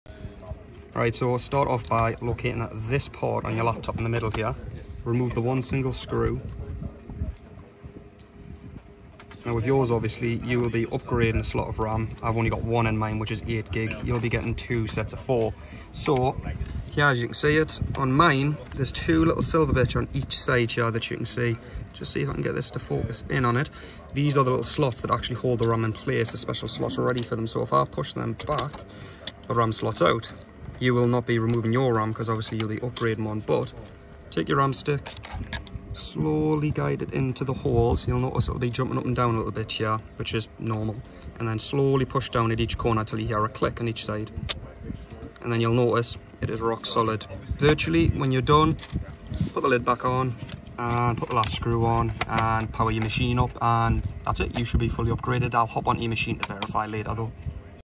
Fujitsu RAM Upgrade
This video is for guiding staff who may have been sent RAM and wish to upgrade themselves

Laptop in Video Fujitsu RAM Upgrade
RAM upgrades are pretty universal the only difference is the location of the RAM per device but inserting the ram will be the same across all devices

only staff who would be doing is is those who missed the 4GB to 8GB RAM upgrade